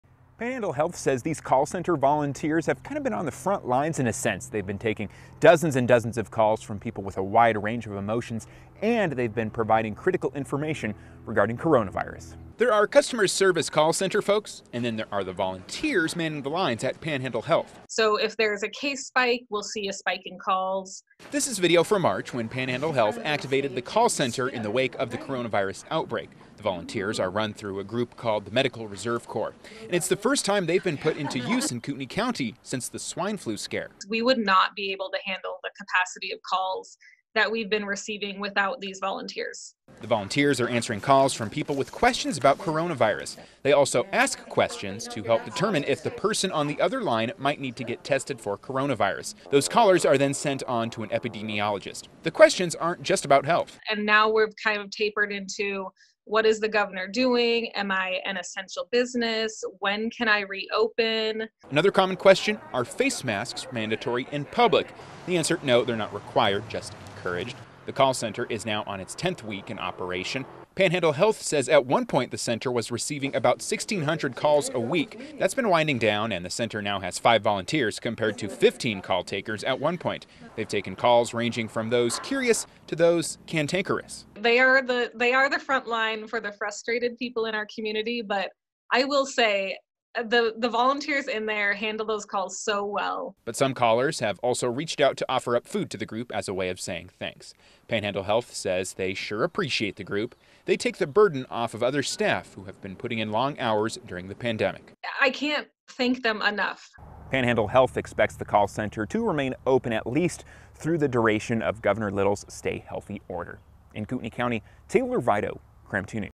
Volunteers help bolster Panhandle Health District's coronavirus call center
The volunteers are run by a group called the Medical Reserve Corps, and this is the first time Swine Flu pandemic.